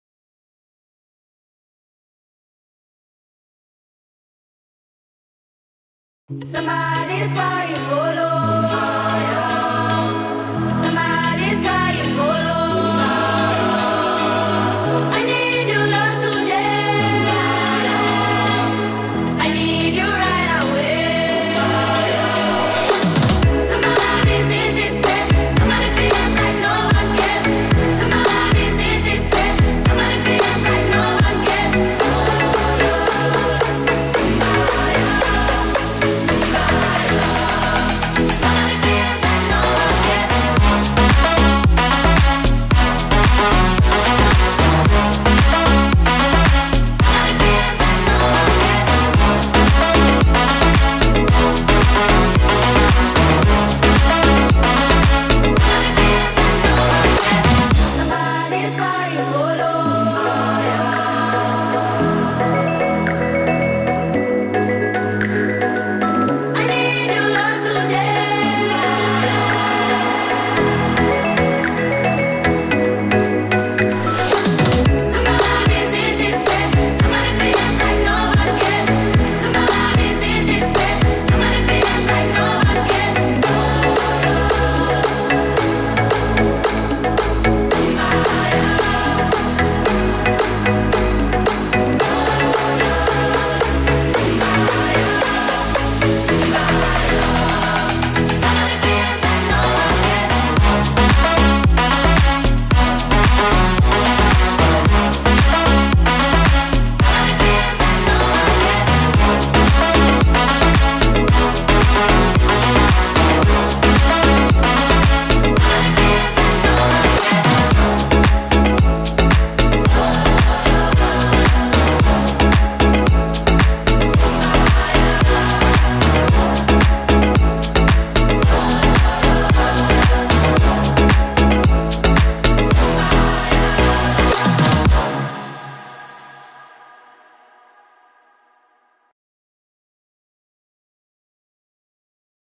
PaxEast 2019 Review One Day Only
I had such a great time at Pax East i decided to put together a quick montage! Boston was also a very lovely place to visit